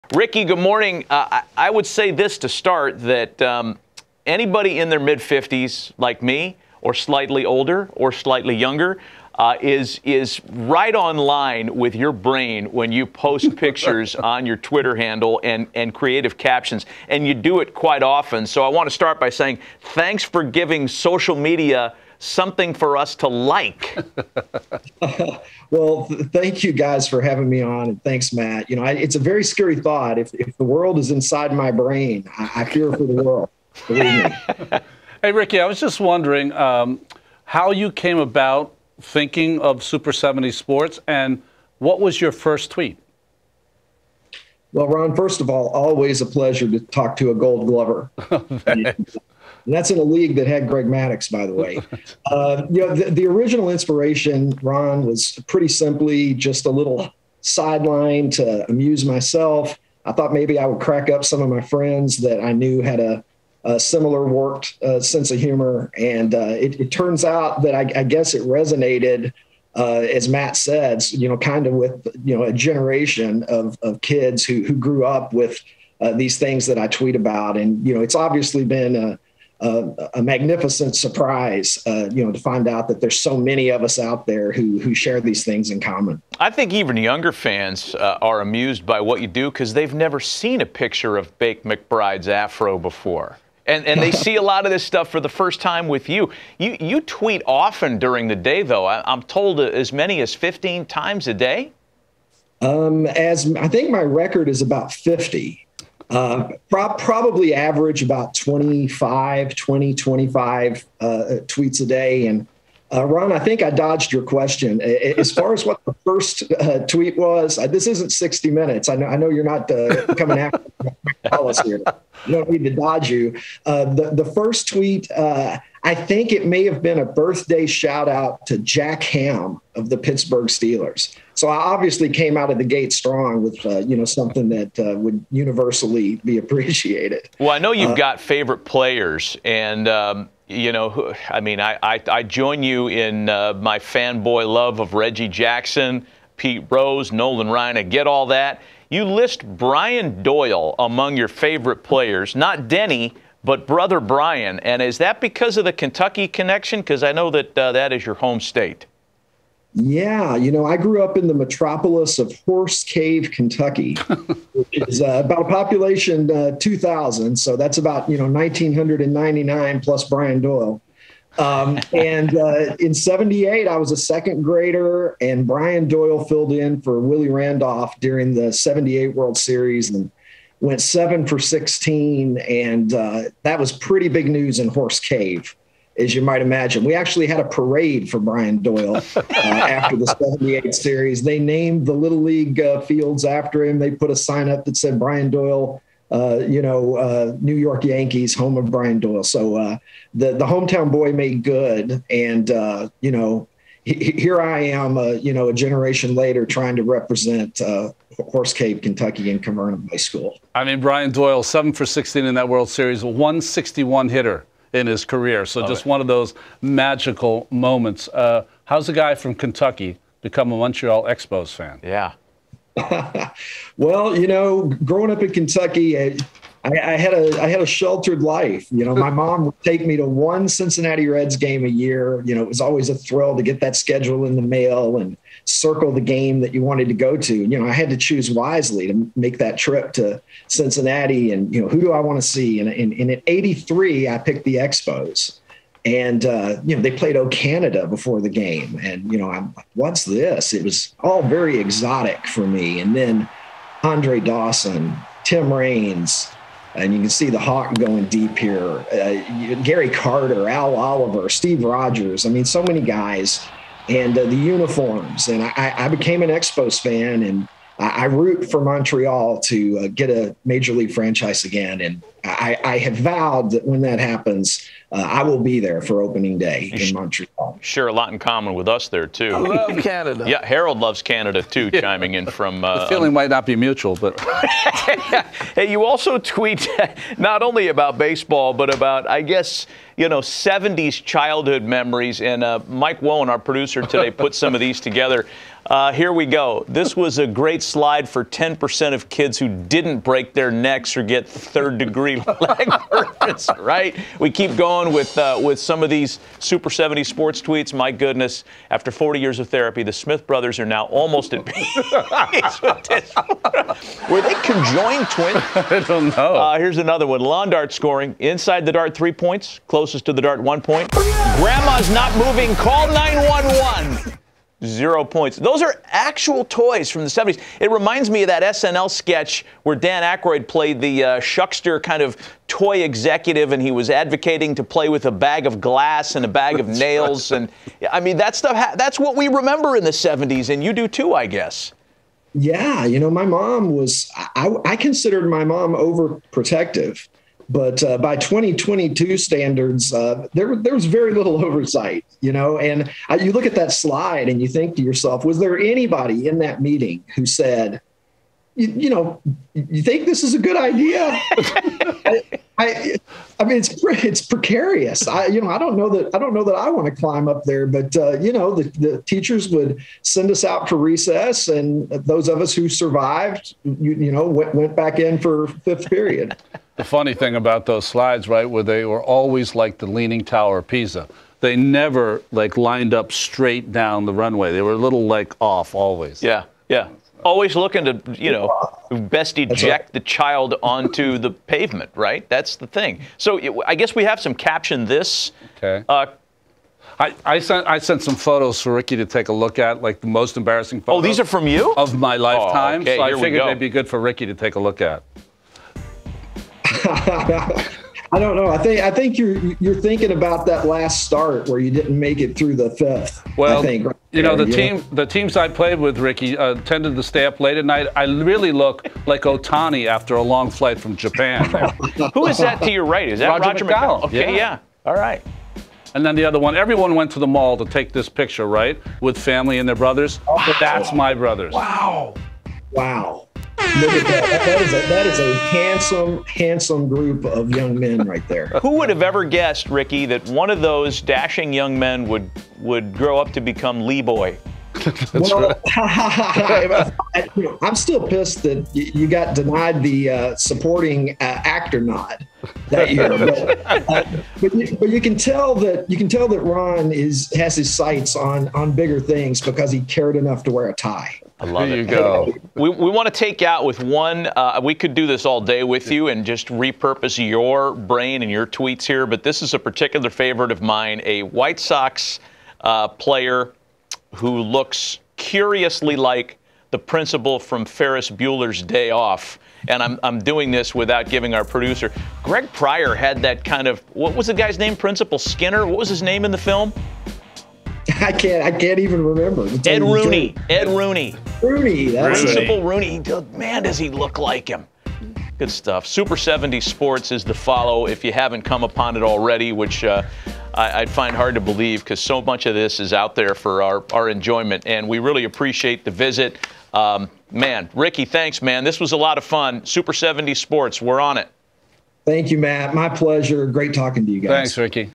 Super 70s Sports Ricky Cobb joins Hot Stove
Ricky Cobb, the brains behind one of the most popular sports twitter accounts, joined us on Hot Stove to discuss his favorite athlete, how he started the account and more.